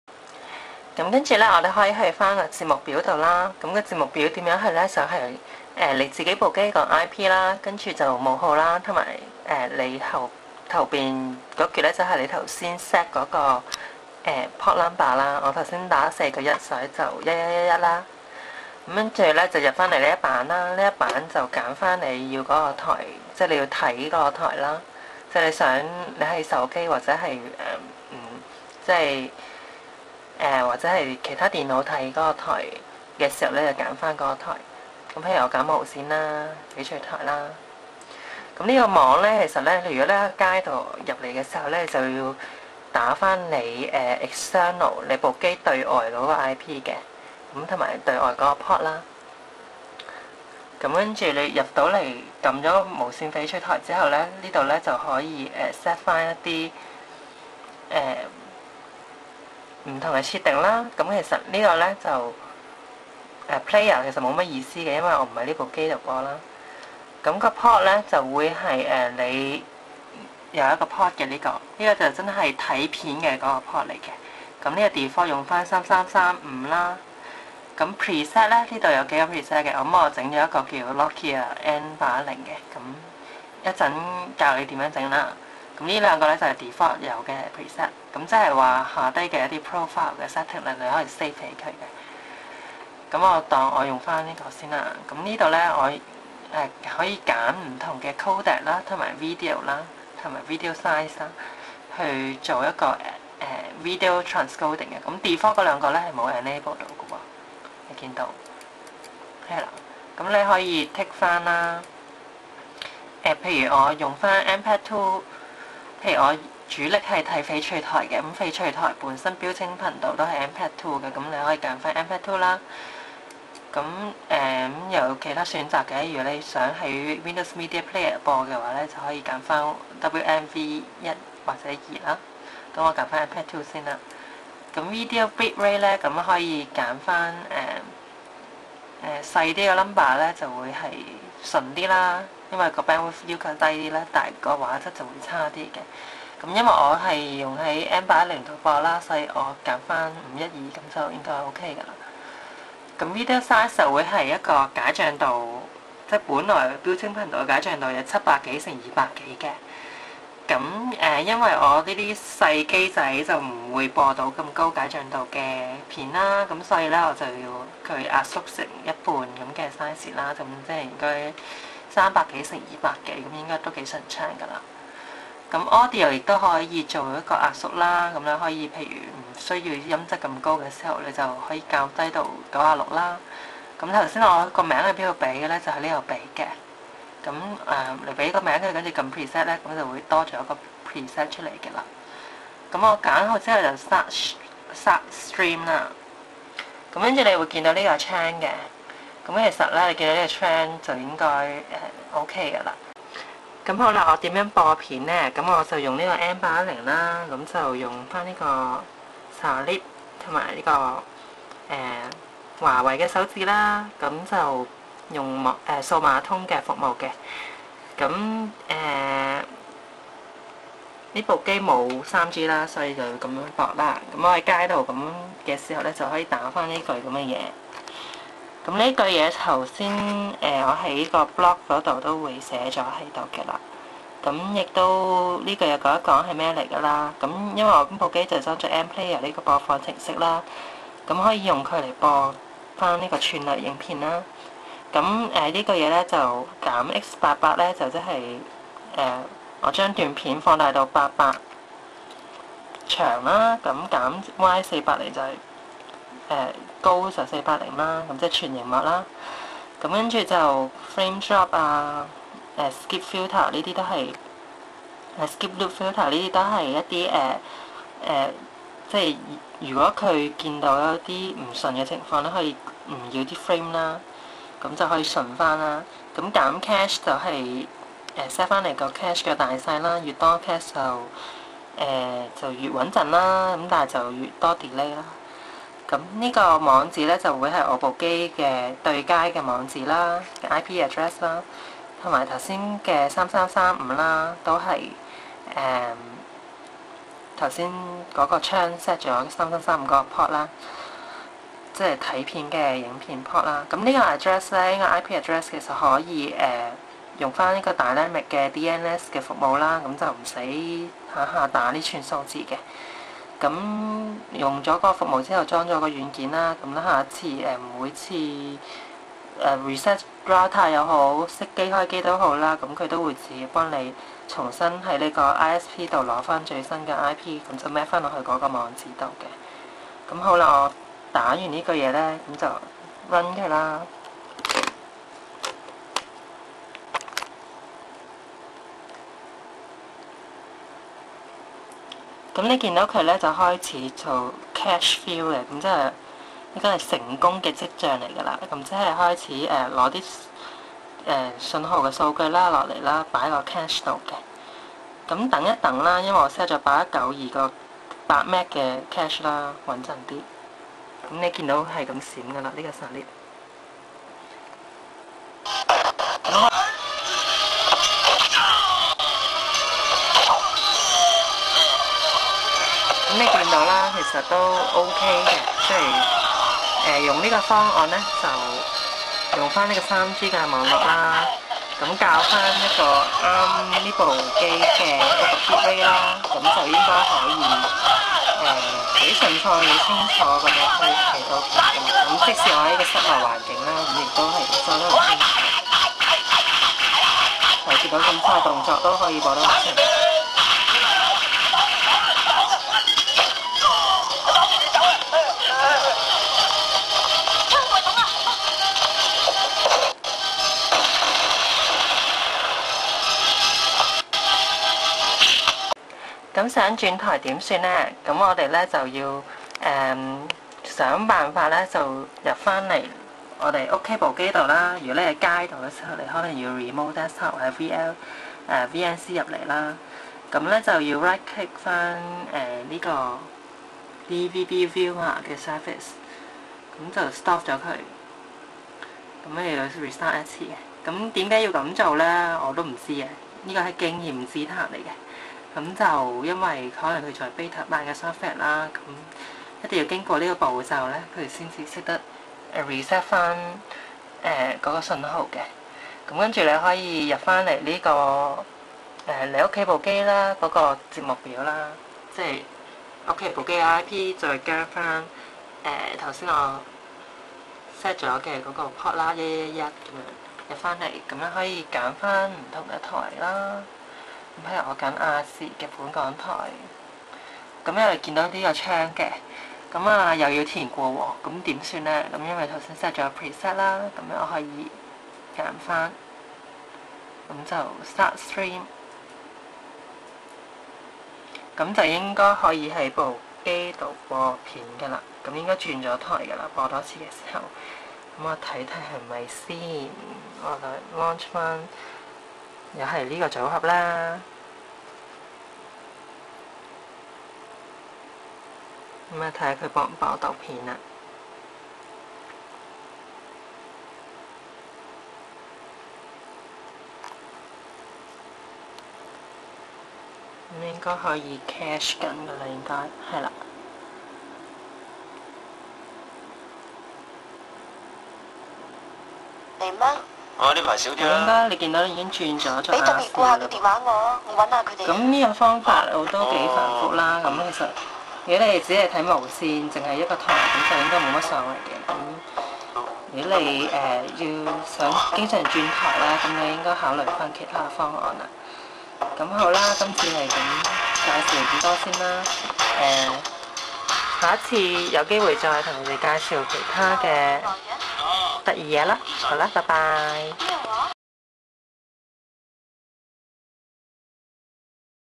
DVBViewer Recording Service + VLC v0.8.6i Digital TV Streaming Part 3
DVBViewer Recording Service and VLC Media Player v0.8.6i Digital TV Streaming Part 3 - Client (player)-side Configuration.